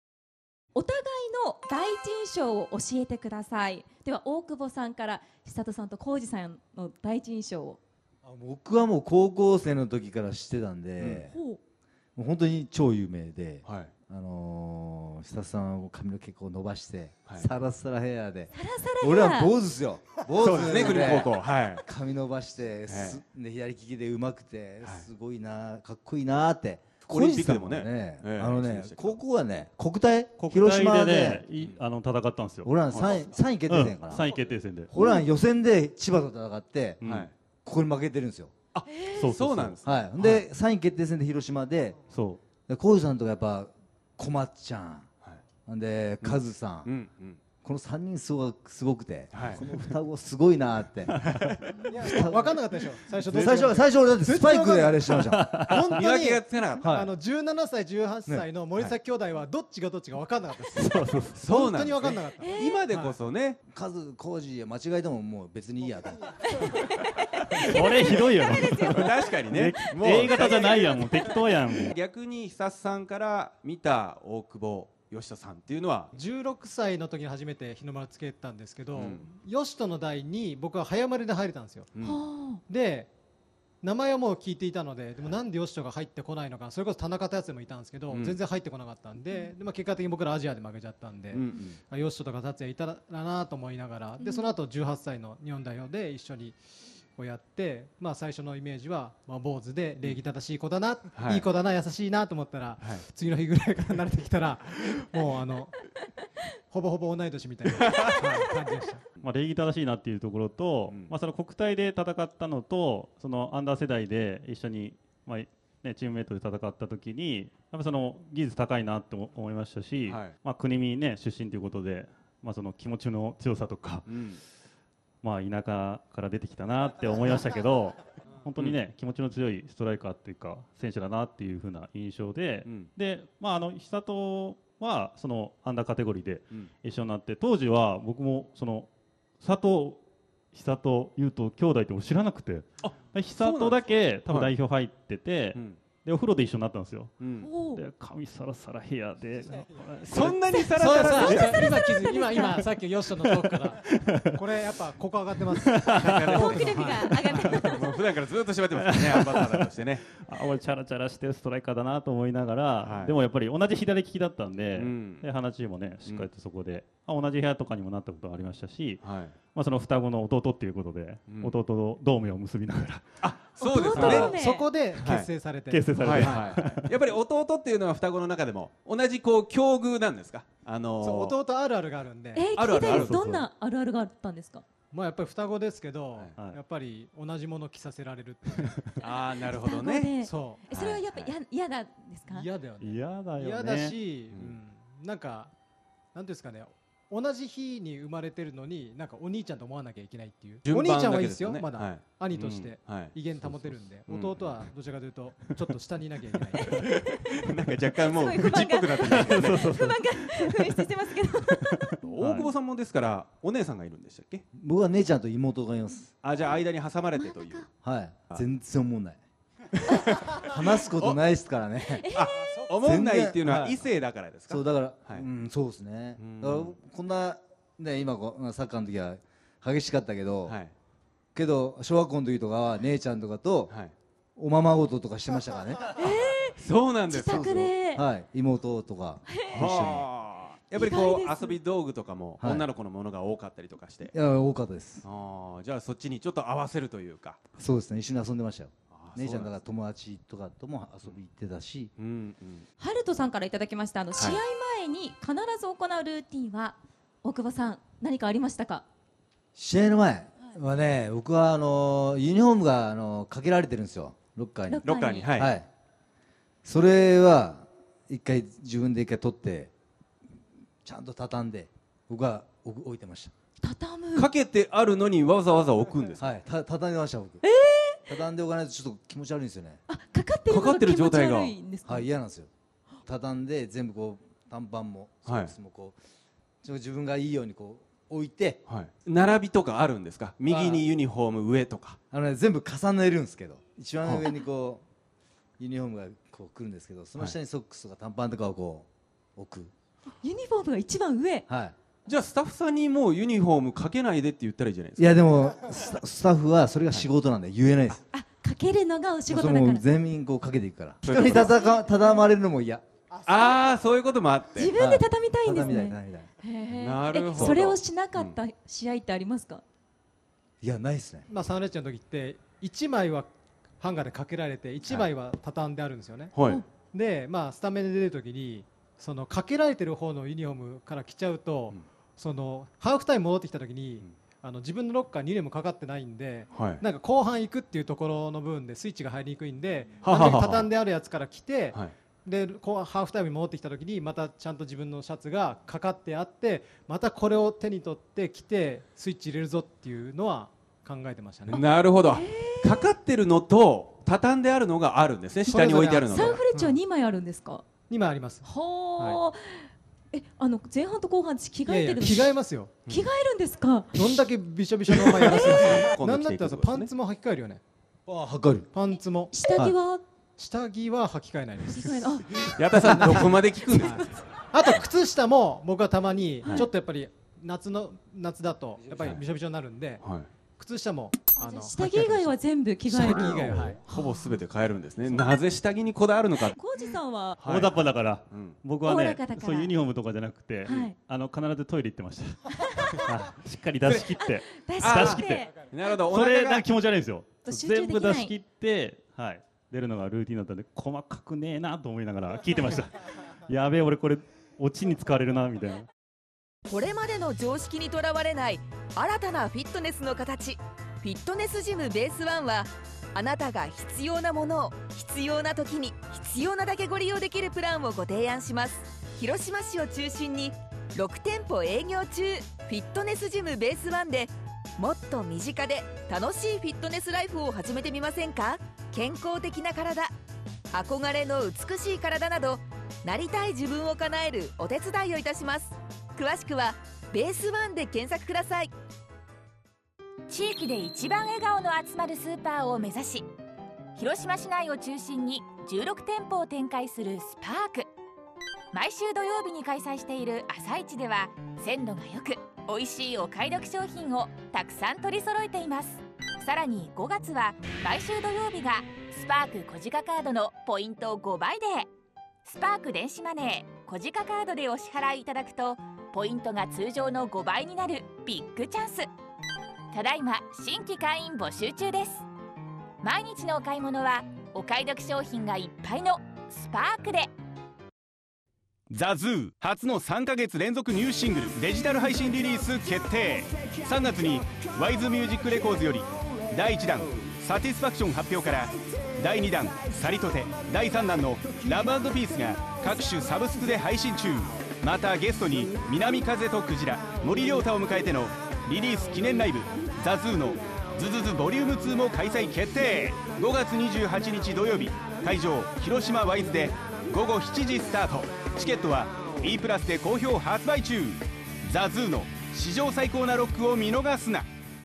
【大久保嘉人＆佐藤寿人】出会いはユース時代…髪伸ばして上手くて凄かった○○＆見分けがつかなかった○○【森﨑浩司】
J１最多得点記録・大久保嘉人さん、Jリーグ歴代最多得点記録・佐藤寿人さん、サンフレ黄金期を支えた森﨑浩司アンバサダーのアテネ世代のレジェンドが初めて出会った当時の思い出を語り合う！そして、現役時代に行っていた三者三様の試合前ルーティンを紹介！

▽地上波でも放送中！
毎月第3水曜日午前0時55分～広島ホームテレビで放送